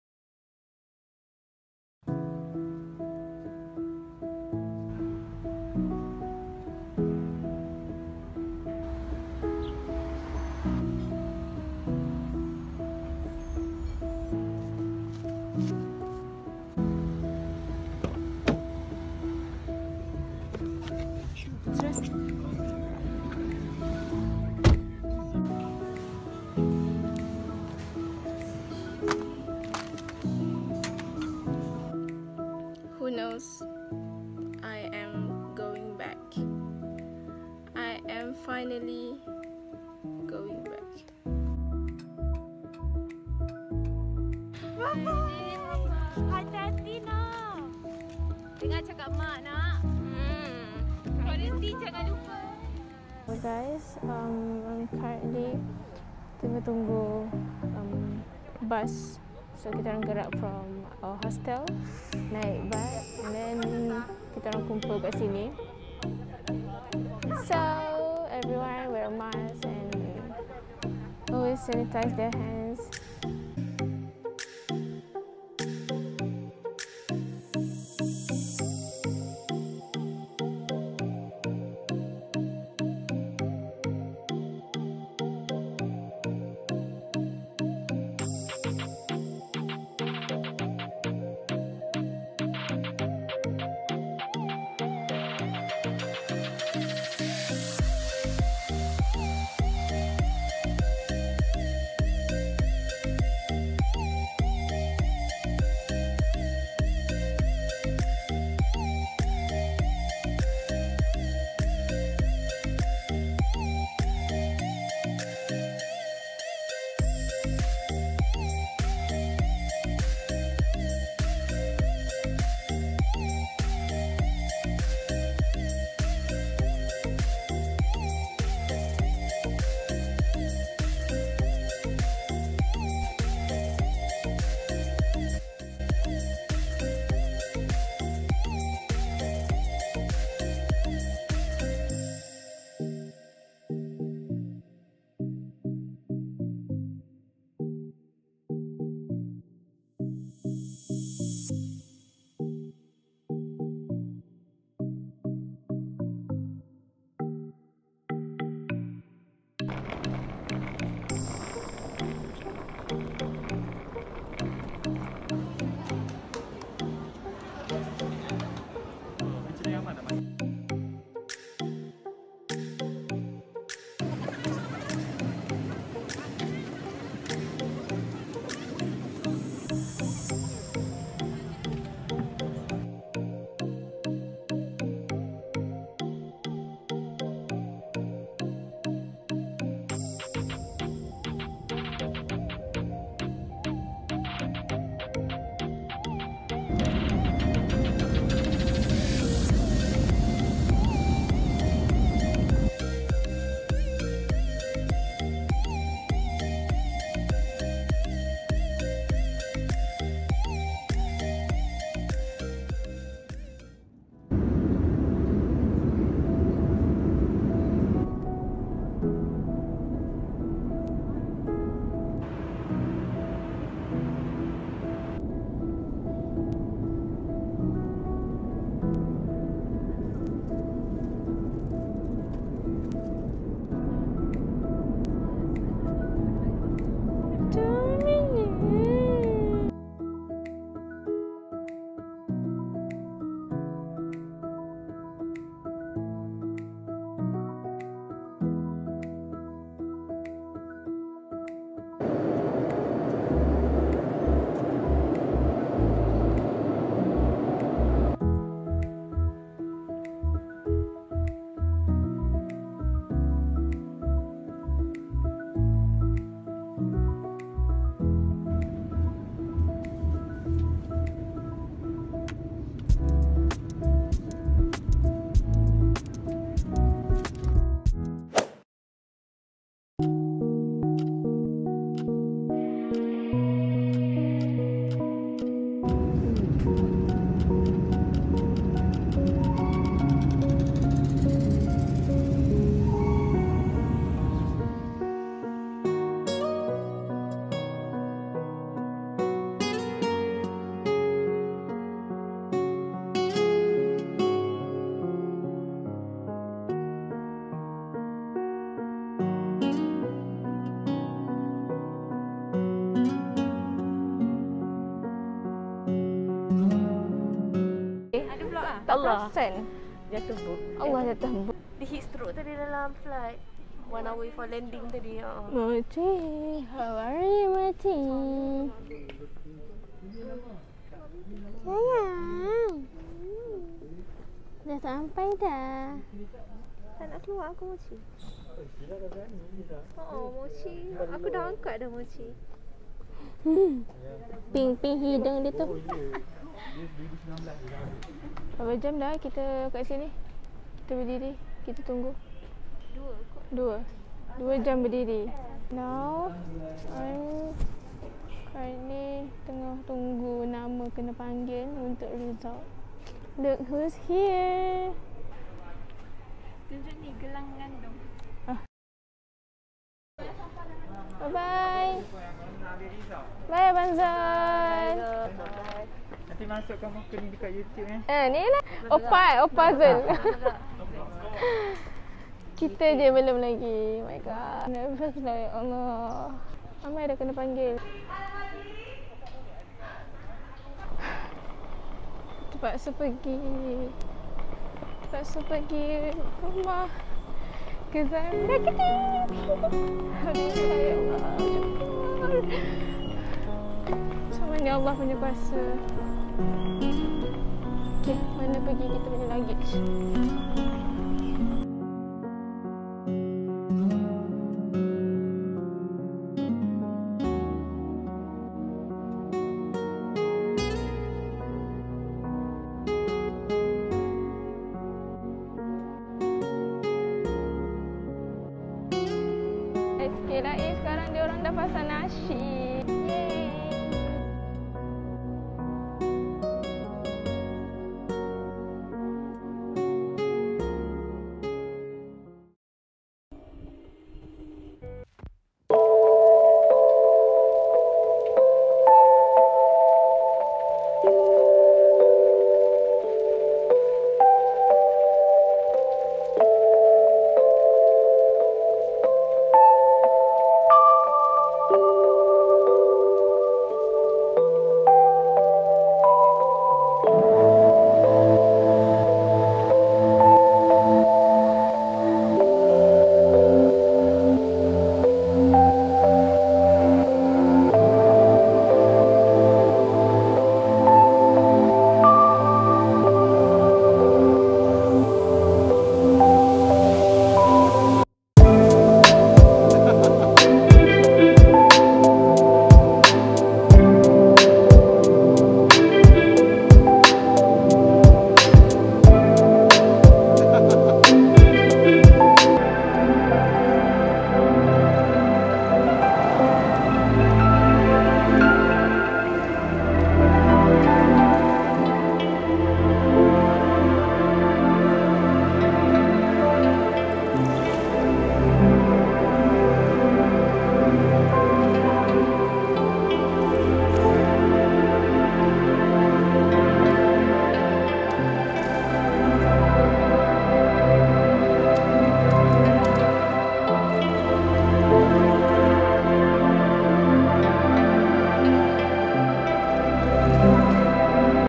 RUSSIA - KUALA LUMPUR | Flying back home Malaysia during COVID-19
Here's the compilation footages that i've filmed along this journey.
To be honest, I was planning to not going back home during the Summer break (Last September). Actually i even told my family in Malaysia that i was totally fine staying in Russia. Eventhough most of my batchmate already went back Malaysia earlier, I refused to join them although the cases were arising everyday crazily. But, thaught of the ticket prices, my exams, and studies.. I decided to not going back and stay with my other seniors in Russia.

But suddenly, something happened. I changed my mind. Definitely it was the very last minute plan. I booked a chartered flight with other Malaysian students 6 days before we flew. Settled all the documents needed and packed my stuffs by myself. Phew.

Anyways, kuddos to those who contributed in making this journey a huge success and bringing us back home safely after facing a lots of cancellations and hardships. May Allah bless all your efforts. Much appreciated ☺️